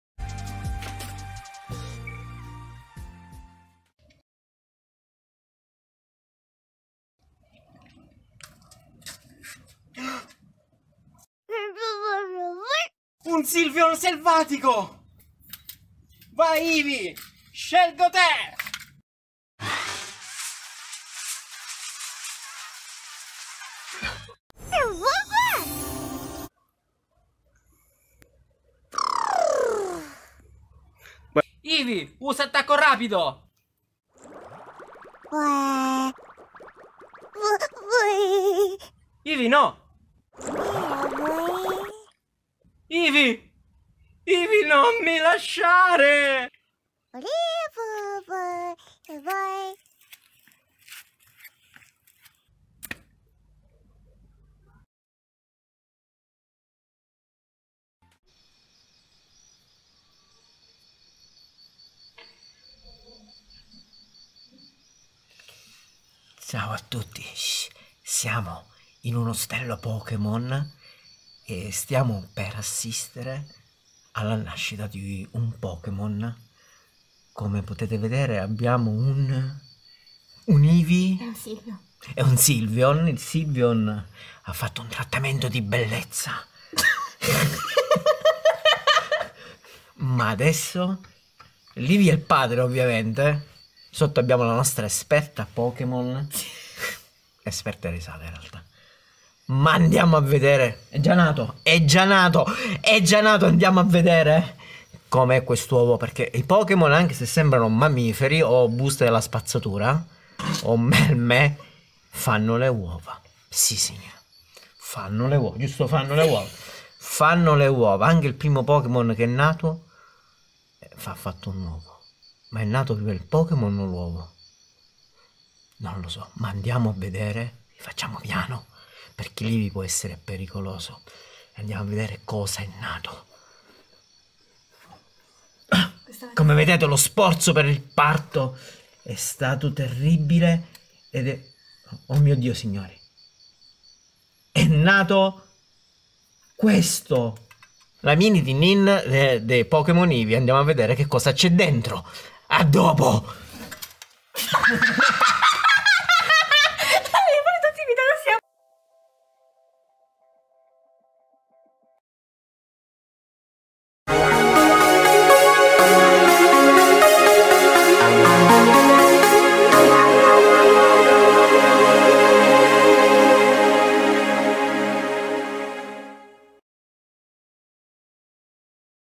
Pokemon nella vita reale - Eevee's lov
Ed ecco finalmente riunite le prime due puntate del live action dedicato al mio amato pokemon Eevee.

Puoi supportare il mio canale ed il mio lavoro cliccando sul pulsante abbonati o facendo una piccola donazione.